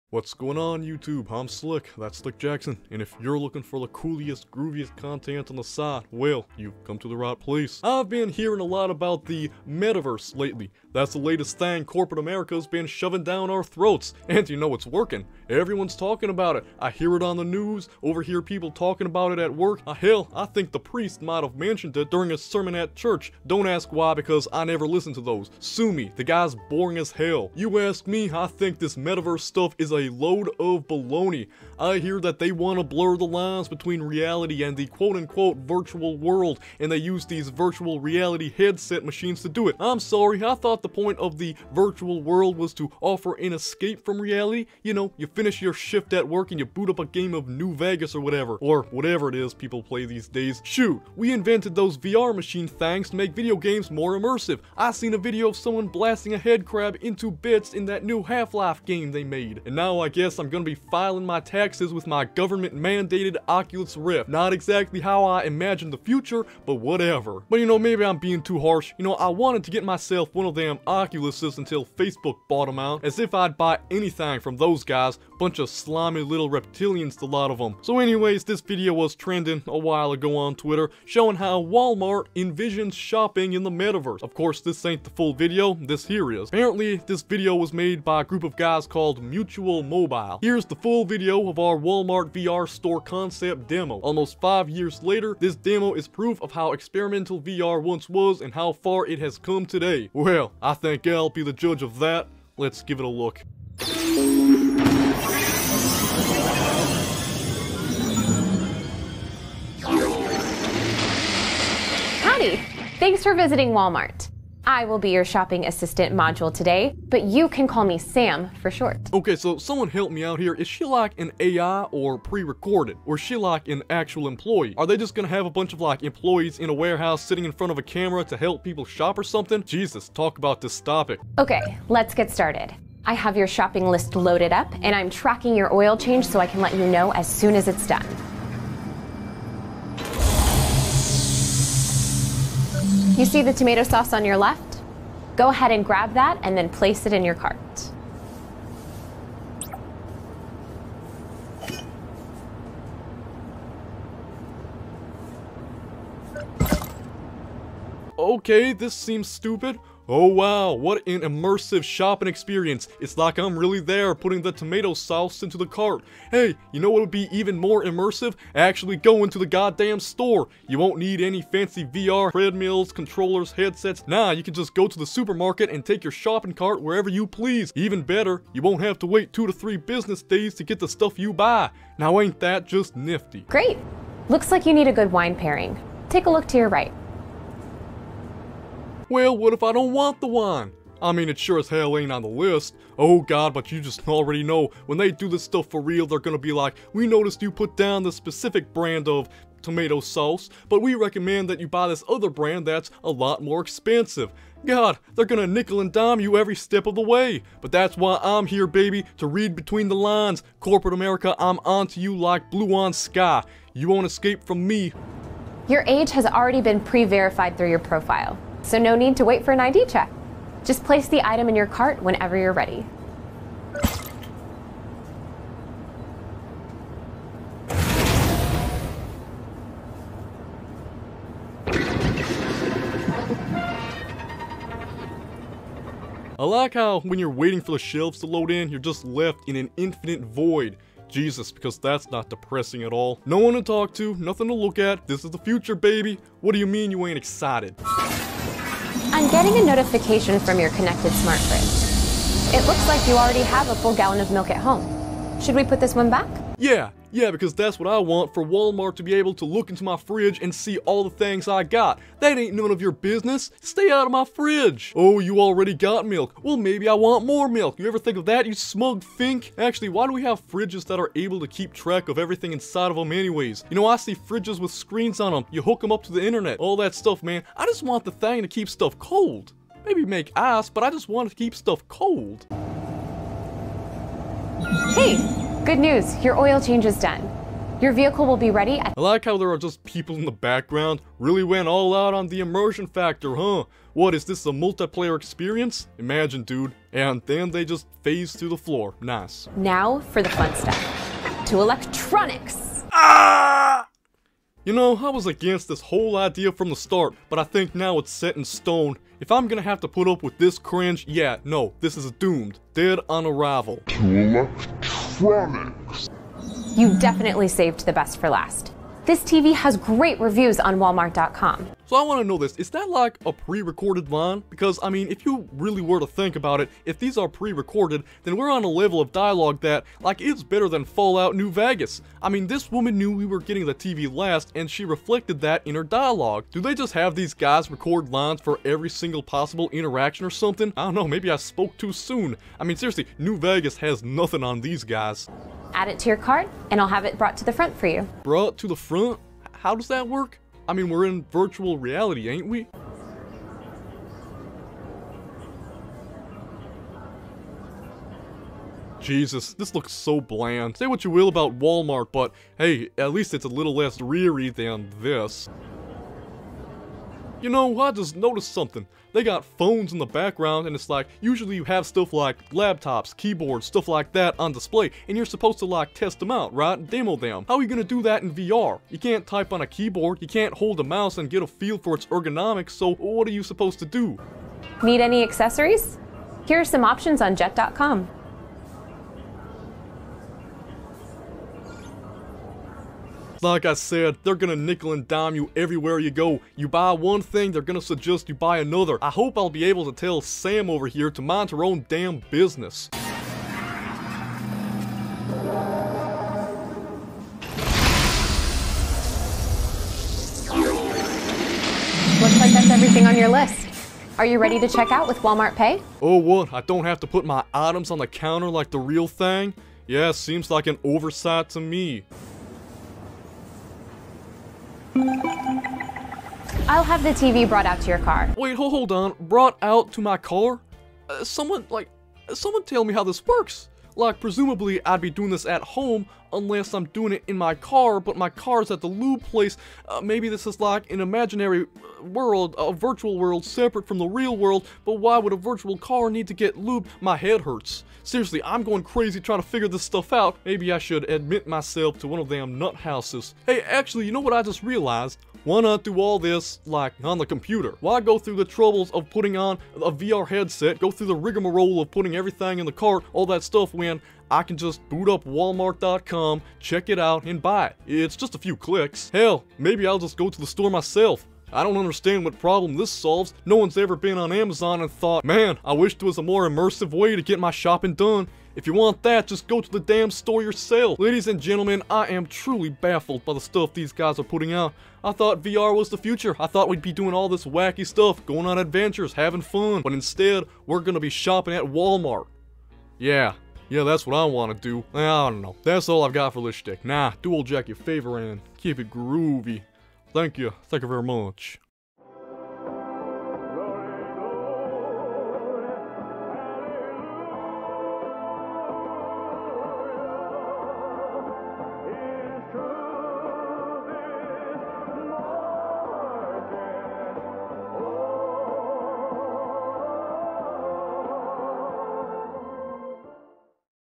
Walmart Wants YOU To Shop in VR! The Metaverse PROVES it's Stupidity!
Walmart envisions that we'll all be shopping in Virtual Reality and the Metaverse. It looks pretty dumb!